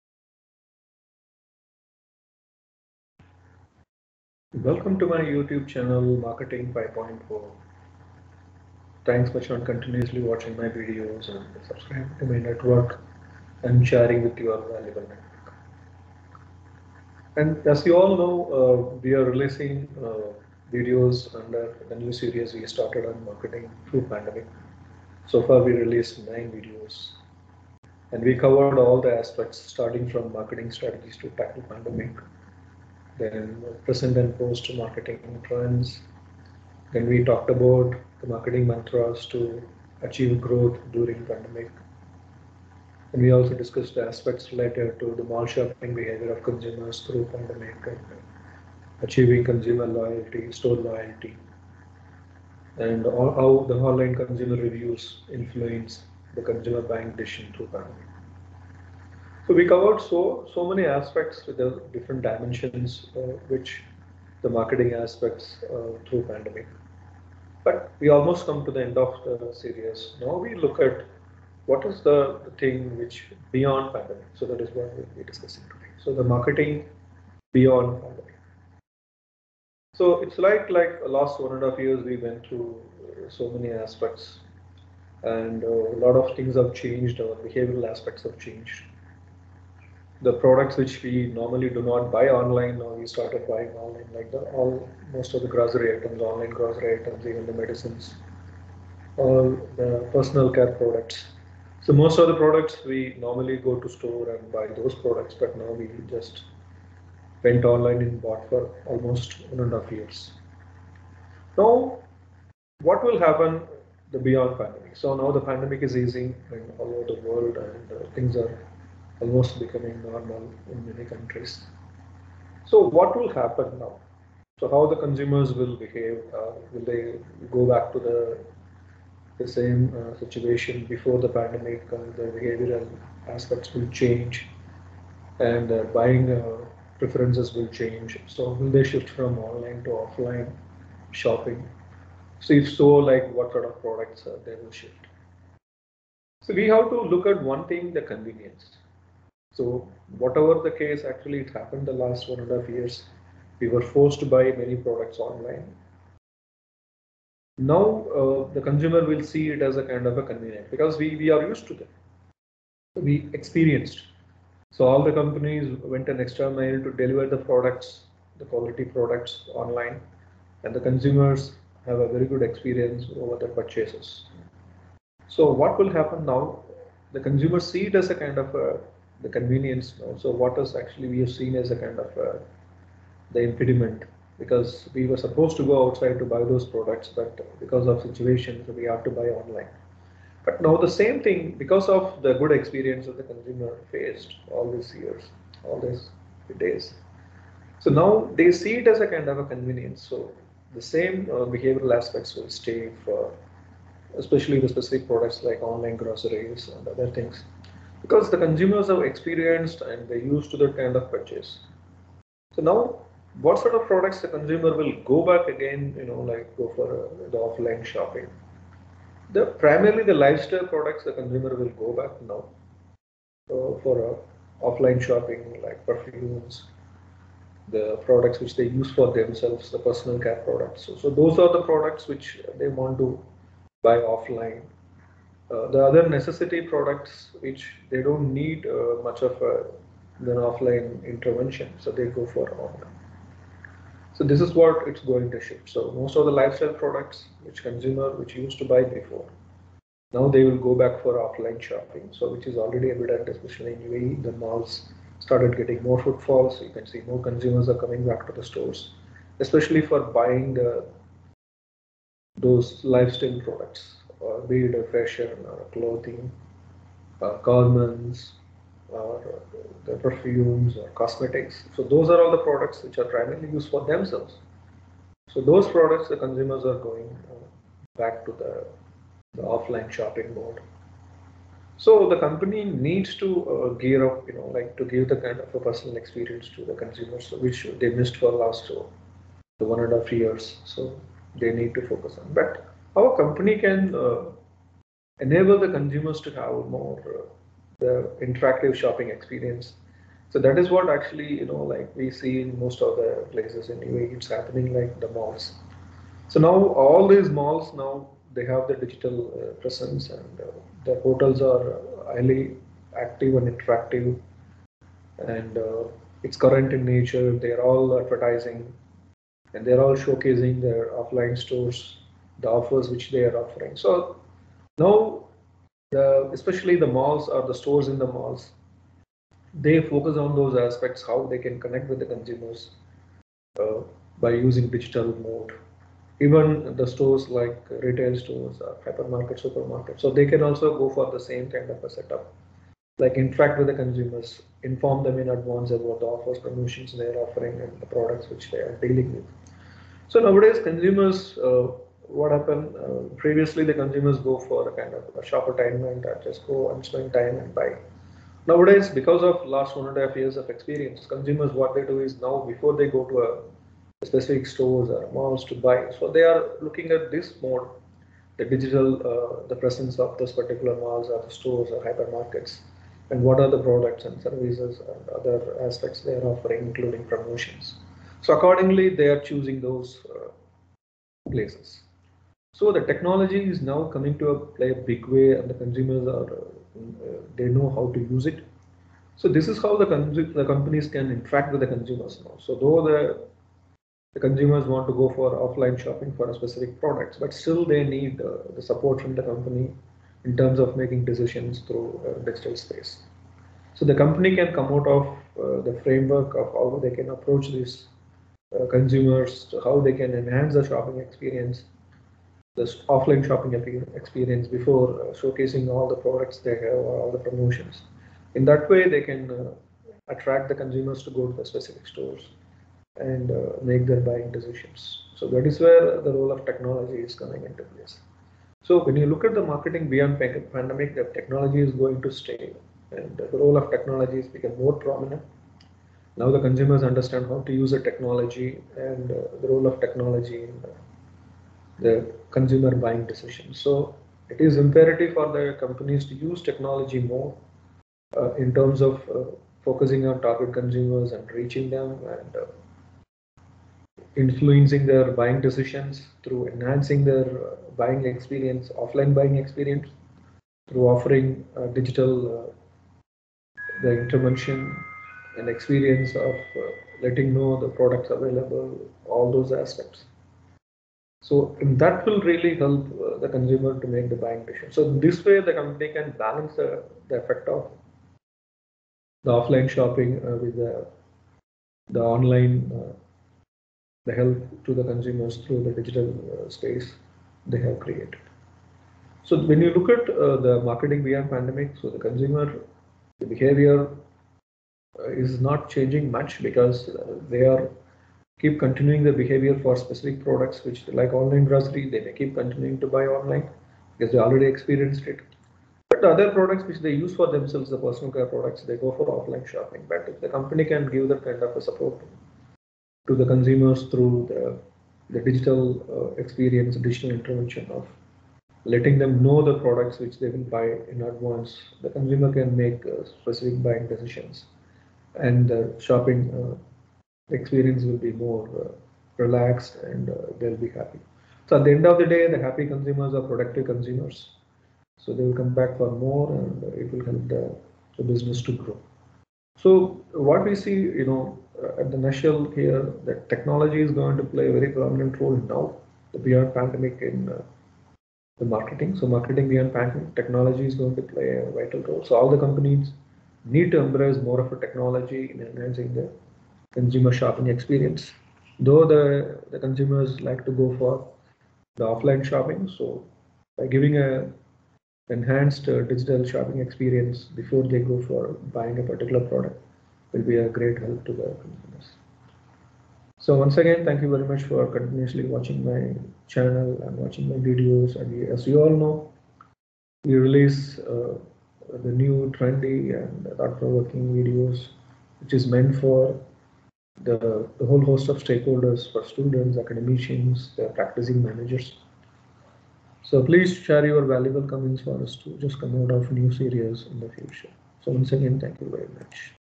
Marketing Beyond Pandemic Expert Speaks
This video portrays the marketing and behavioral aspects of consumers that companies need to focus beyond pandemic as a part of new series  'Marketing through Pandemic - Expert speaks'